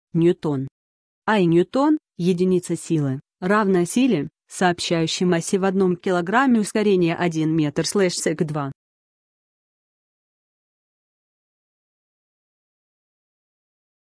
НЬЮТОН - что это такое? значение и описание
Хочешь знать что значит НЬЮТОН? тогда слушай до конца. n n -а и НЬЮТОН, -а, м. Единица силы, равная силе, сообщающей массе в 1 кг ускорение 1 м/сек2. n n Больше слов